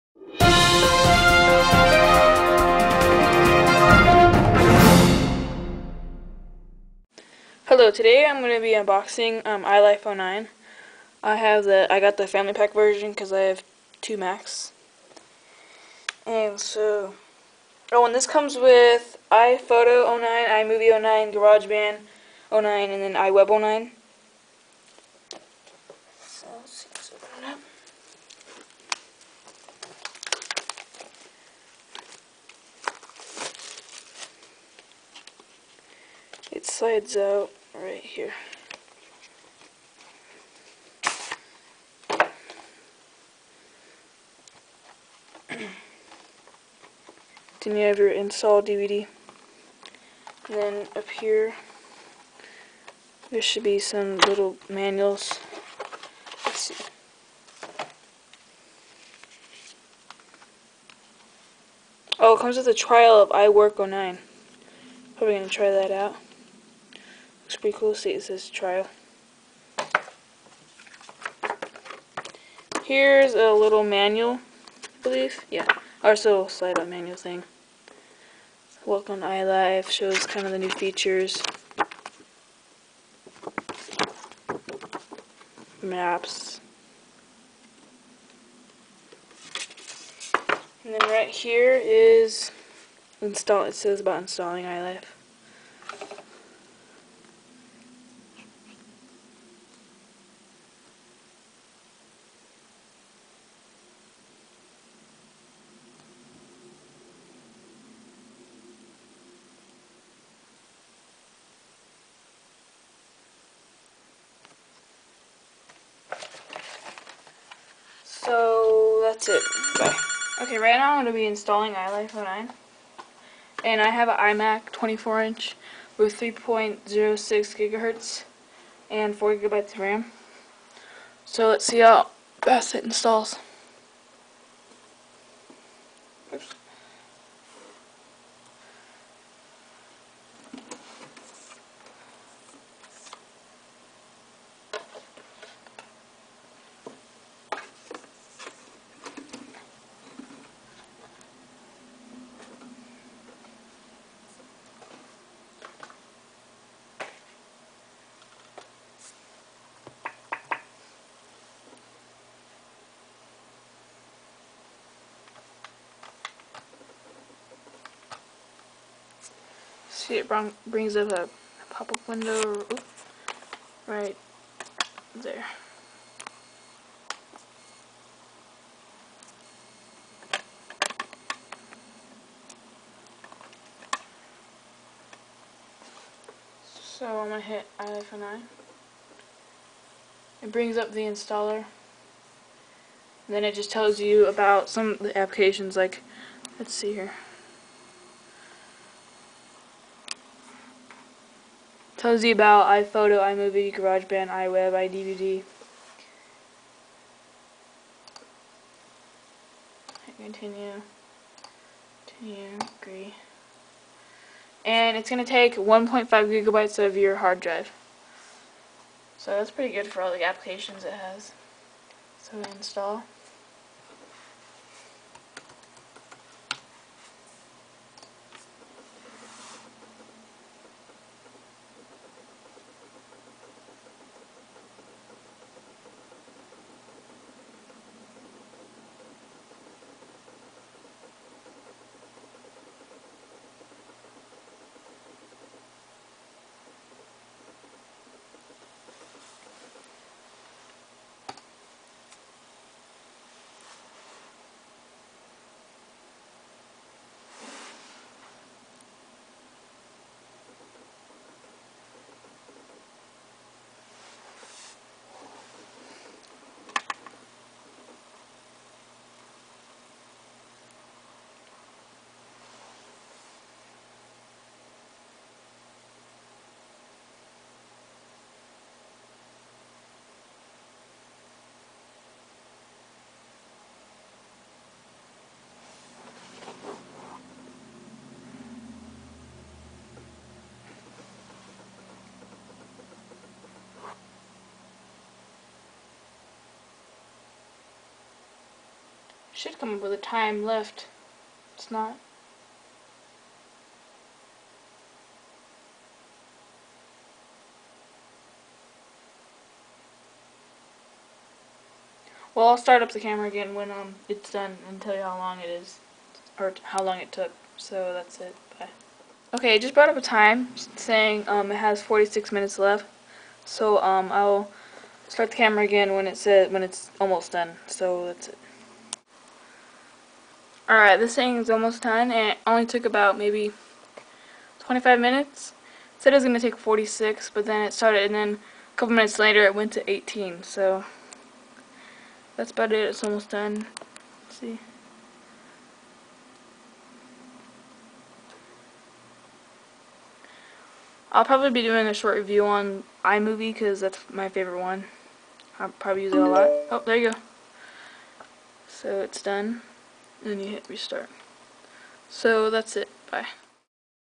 iLife 09 Unboxing
This video is of the new iLife 09. I got it Right before I unboxed it. Also I installed it on my iMac, my iMac has a 3.06 GHz Intel Core 2 Duo Processor with 4Gb of DDR2 SDRAM. I edited this with iMovie 09. I will probably be doing a review on the new iLife sweet.